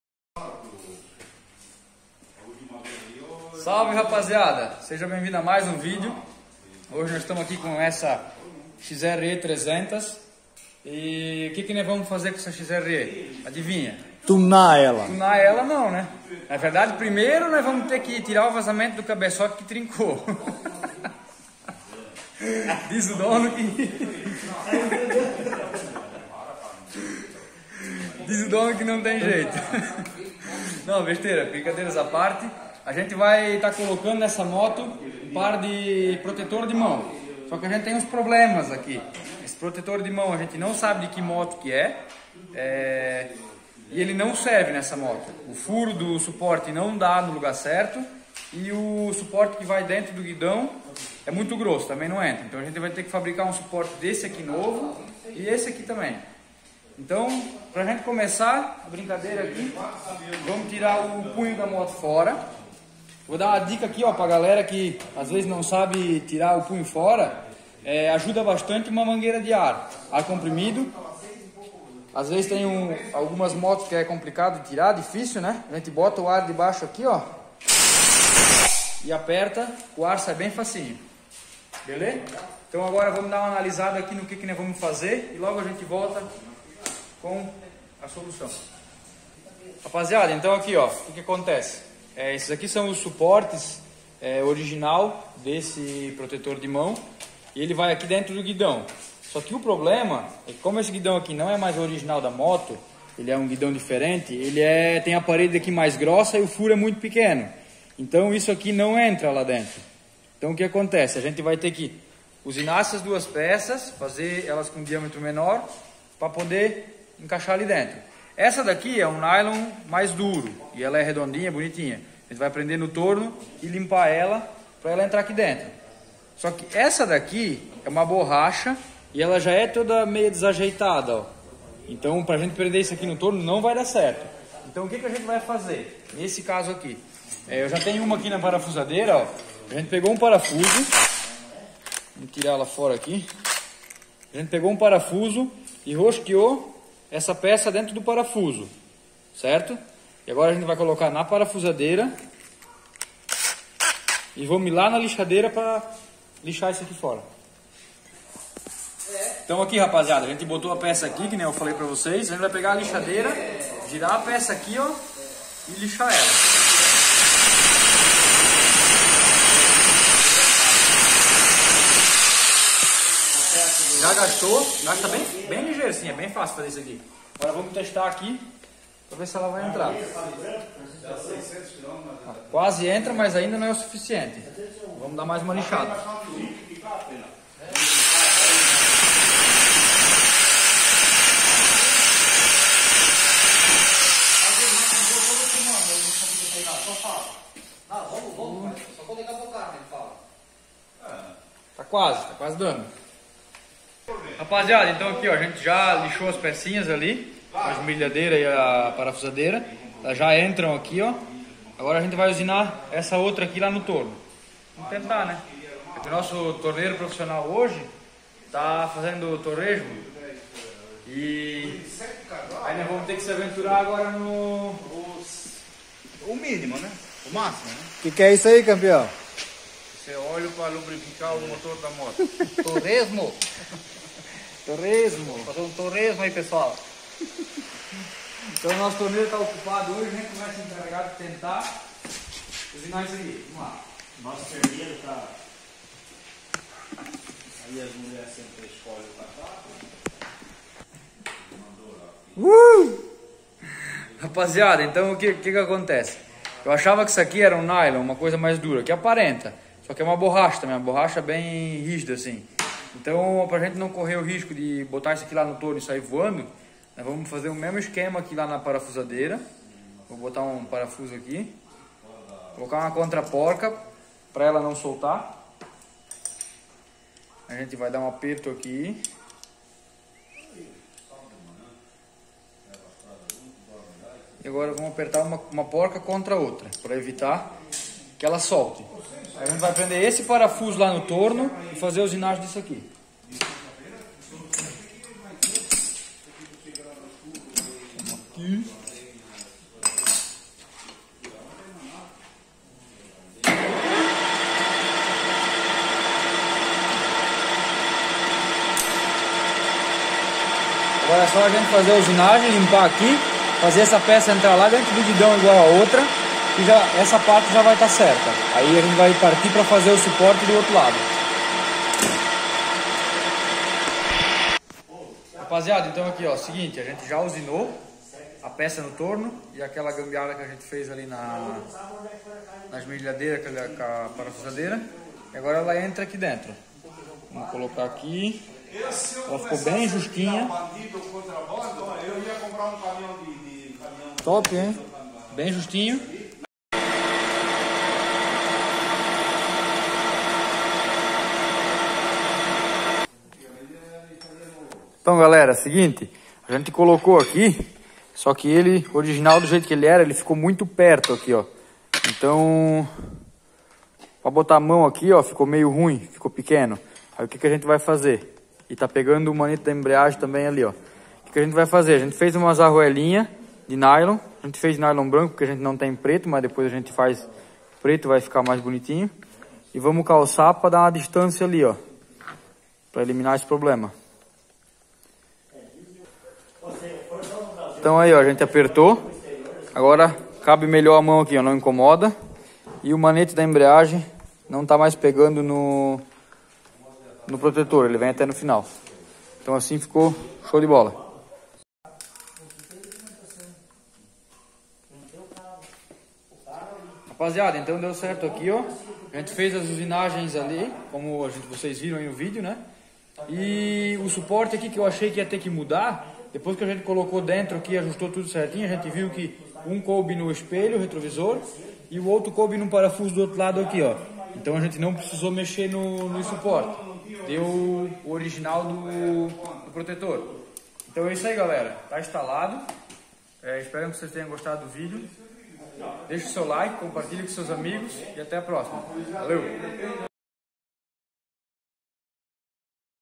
instalação protetor de mão na XRE 300
fala pessoal

vídeo mostrando a instalação de um protetor de mão na Honda XRE 300. foram feitos algumas adaptaçoes para que fosse possível a montagem.